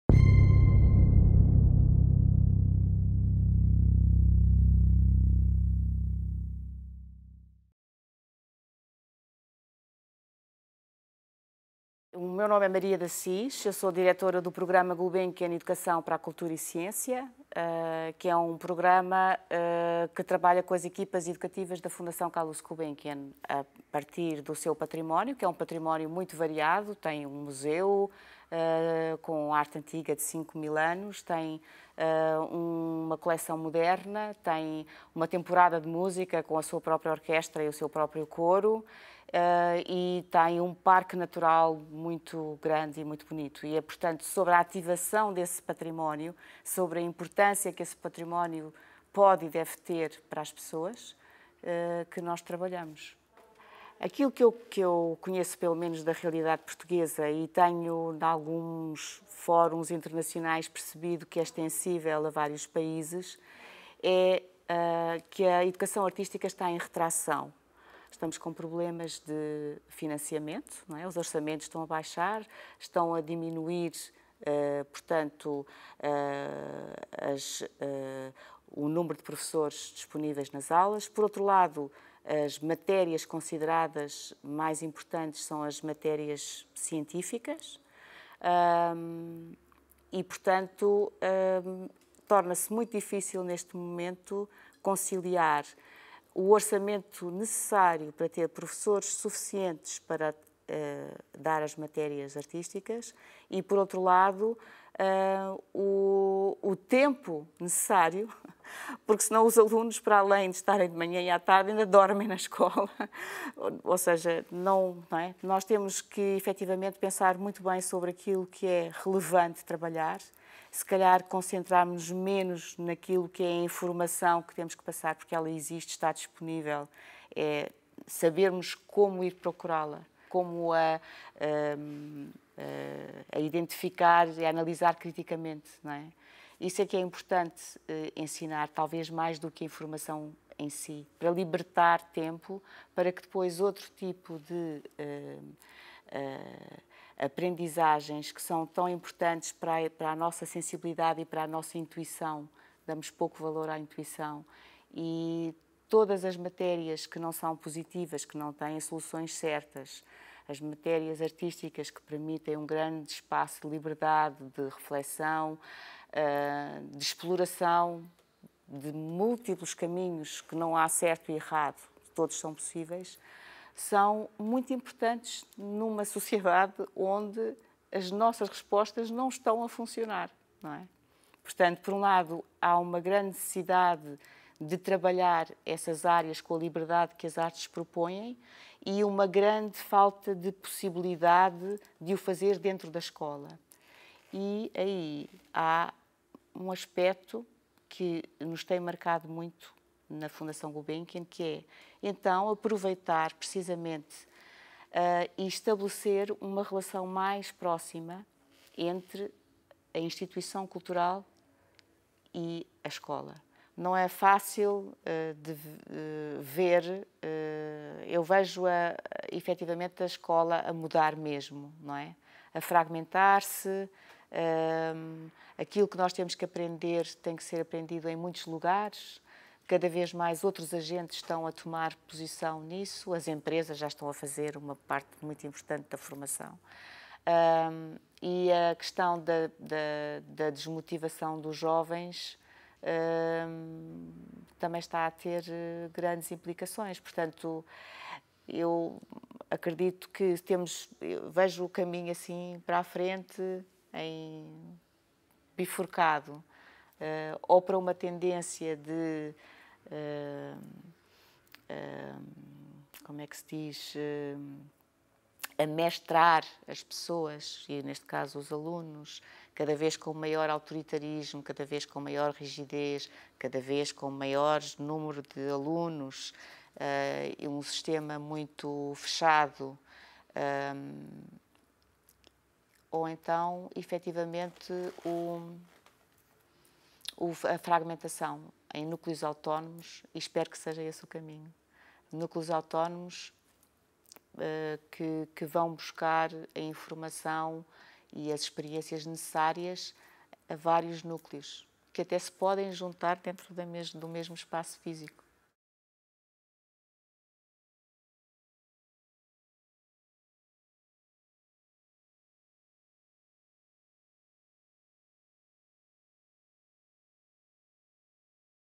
L'educació de les arts · Maria de Assis · Interfícies · Arts Santa Mònica
Prospeccions i alternatives de futur en l'educació artística

Taules de debat al voltant de l’educació artística, del seu paper a l’educació i de la innovació educativa, així com dels reptes de futur per a aquesta matèria

Maria de Assis: Fundació Gulbenkian, Lisboa. Projecte 10x10